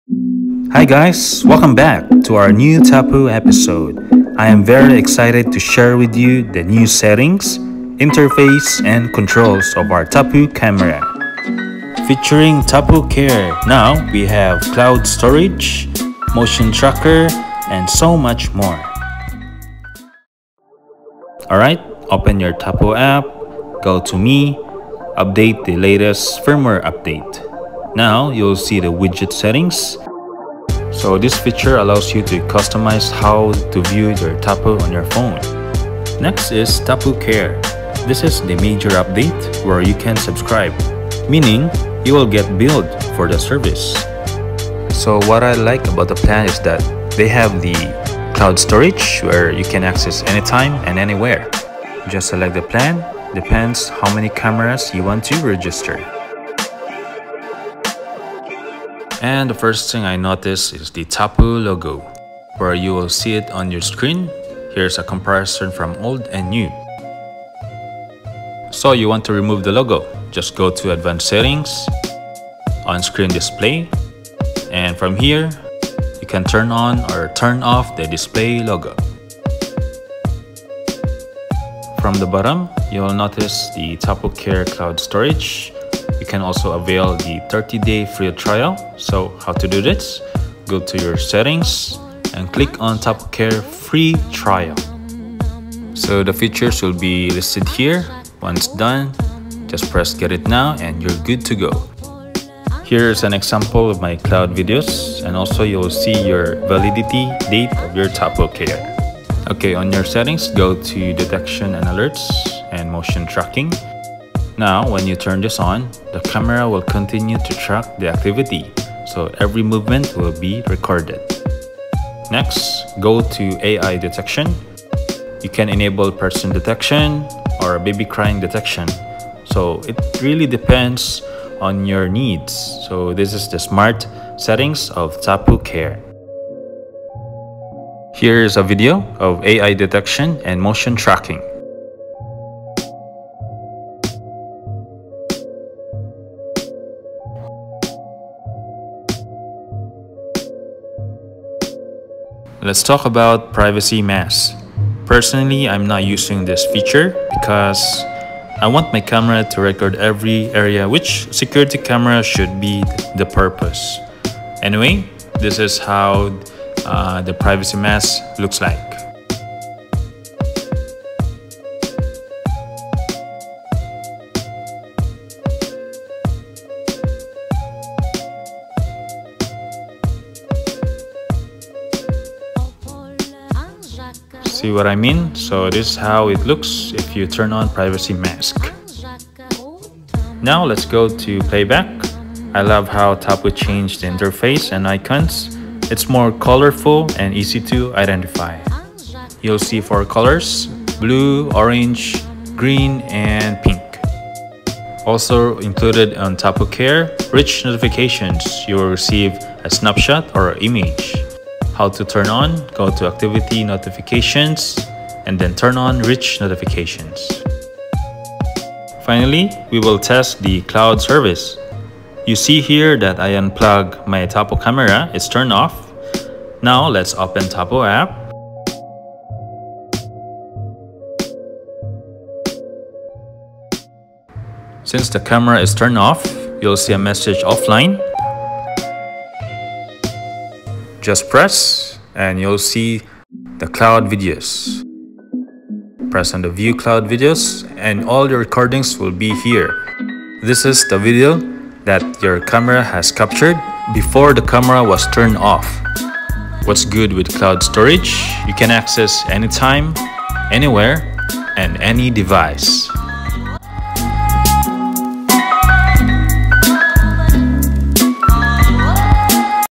New Tapo Care - Cloud Storage, Motion Tracker, New Features (Tplink) Tapo Security Camera
New Features!! Introducing Tapo Care - Finally! Cloud Storage, AI detection, Motion Tracking, New Icons and Interface, Rich notifications and a lot more.
Make sure to update your firmware on Tapo App. Versions 1.0 onwards.
Tapo Care has free 30 days trial so try it now.

Buy Tapo C200 Here.
Amazon best buy (most reviews)

0:00 Intro
0:27 Widgets
0:37 Tapo Care
1:09 Add/Remove Tapo Logo
1:30 Free Trial 30 days
1:50 Motion Tracking
2:02 AI Detection
2:33 Privacy Mask
3:09 New Playback
3:25 Rich Notifications
3:39 Cloud Storage/Videos

Special thanks,
Musician: Not The King
Musician: Jeff Kaale

Thank you everyone for your support. You are all AWESOME!!